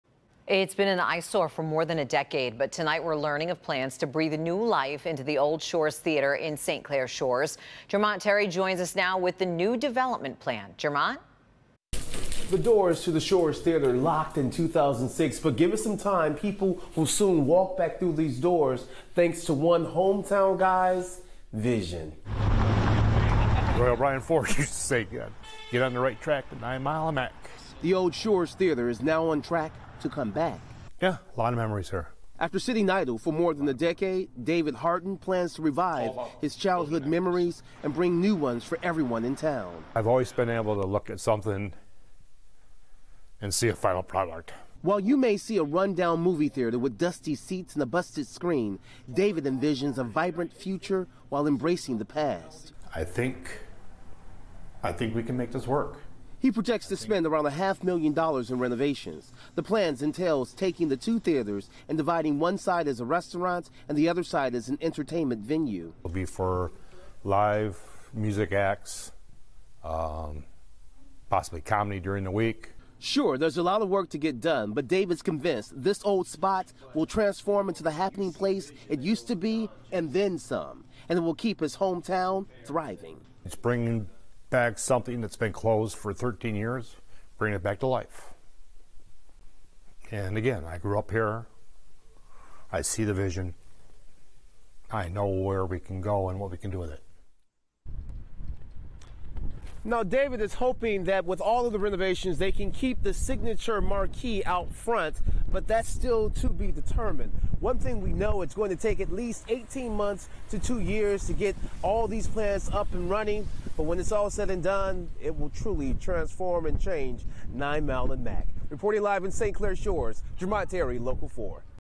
Vacant Shores Theatre in St. Clair Shores to be renovated
A theatre in St. Clair Shores is going to be renovated.